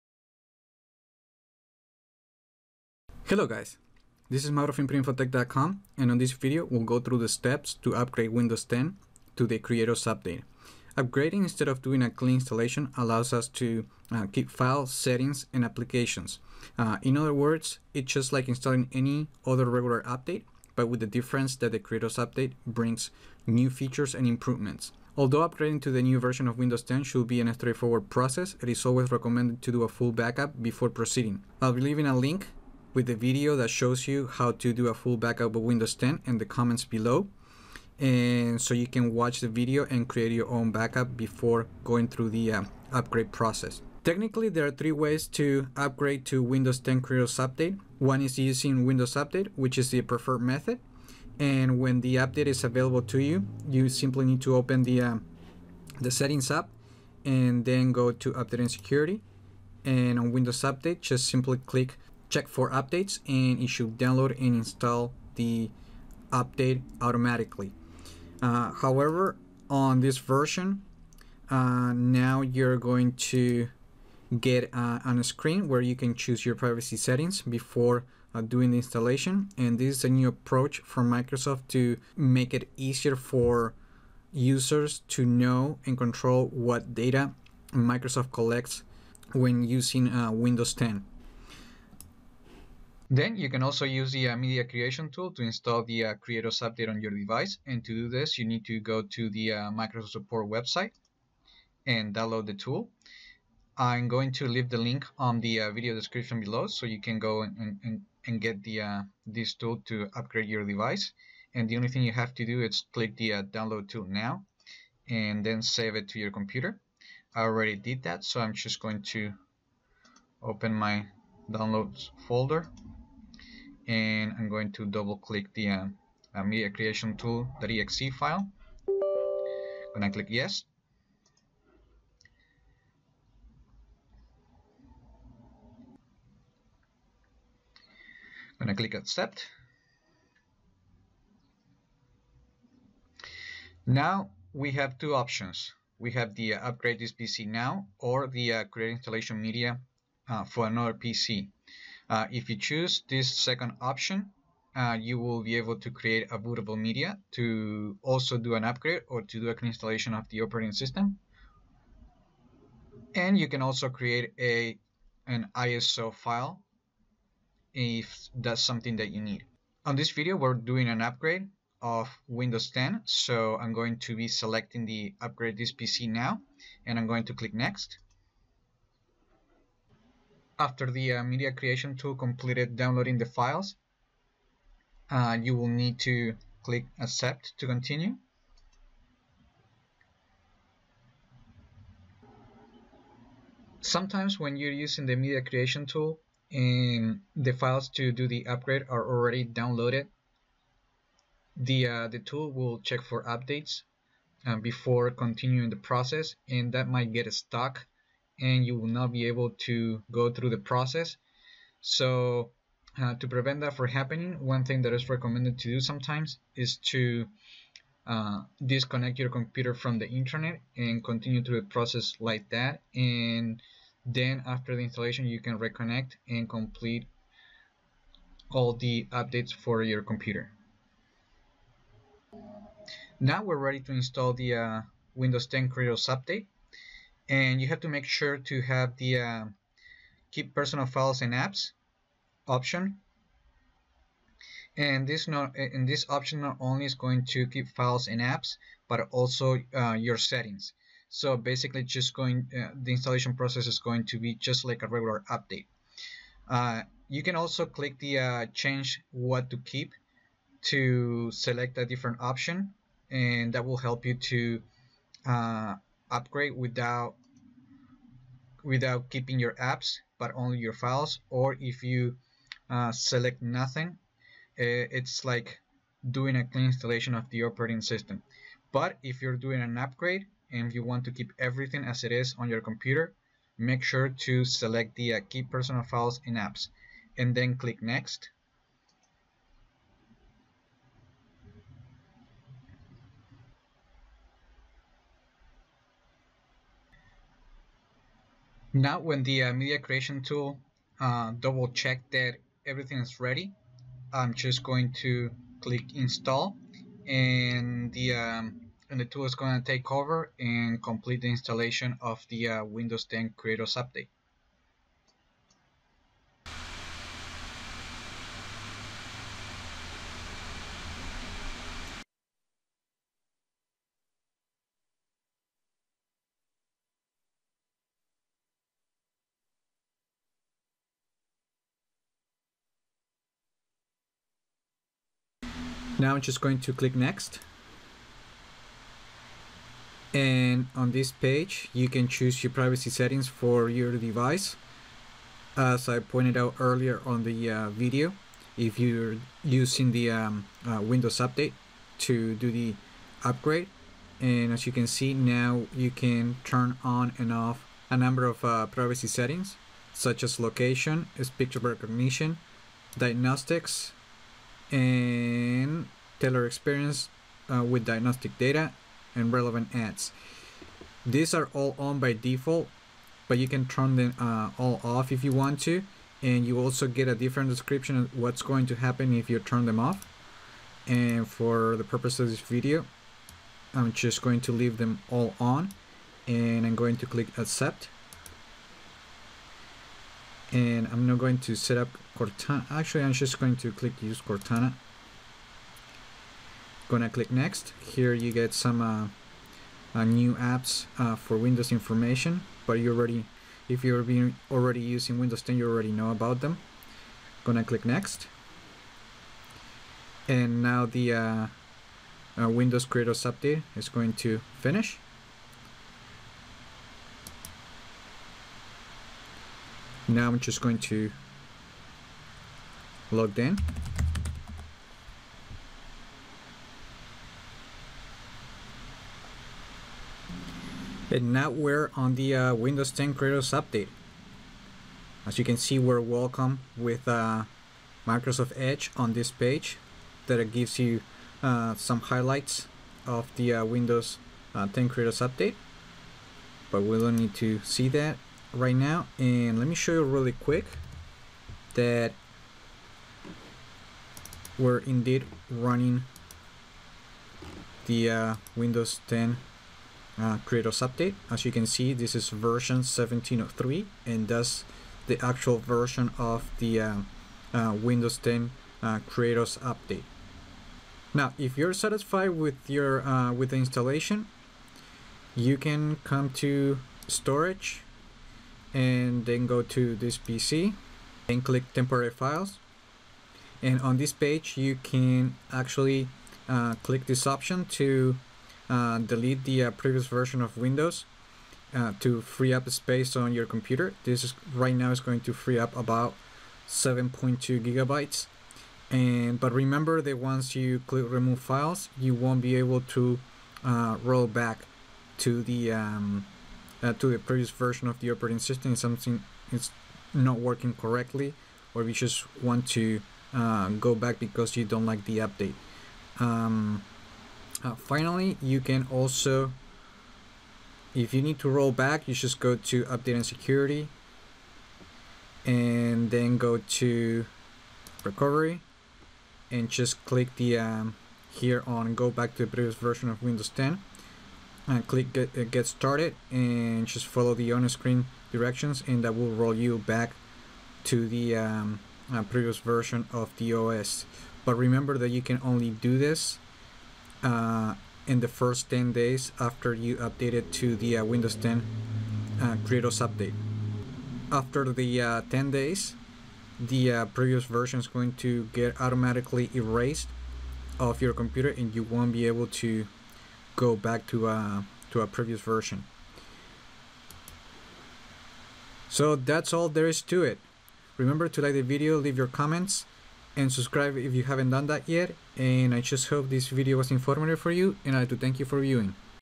Windows 10 Creators Update: upgrade process
Use these steps to upgrade to the Windows 10 Creators Update (version 1703), and learn to go through the new out-of-box-experience (OOBE) to complete the installation.

Note: Windows 10 Creators Update will release in early April 2017 (reportedly on April 11th). If you use the Media Creation Tool before the official release, you won't get Creators Update.

Note2: When in the video I say there are three ways to upgrade, I meant using Windows Update, in-place upgrade using the Media Creation Tool, or using the tool to create an ISO file or bootable media.